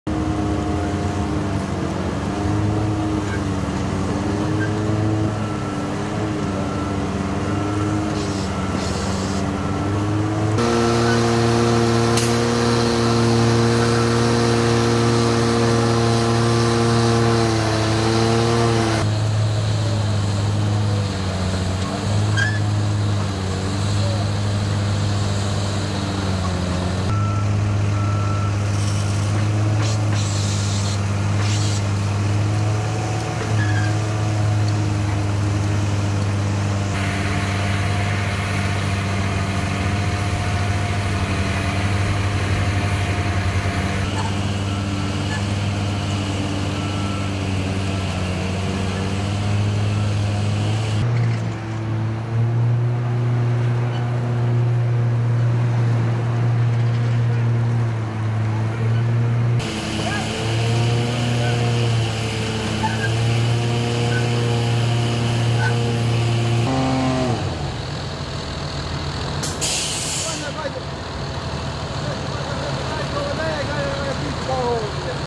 raw video of the pothole killer
Mother Nature took a toll on roads across the Northeast this winter. Bridgeport is pushing back with the Pothole Killer. Check out raw video of the mean machine in action.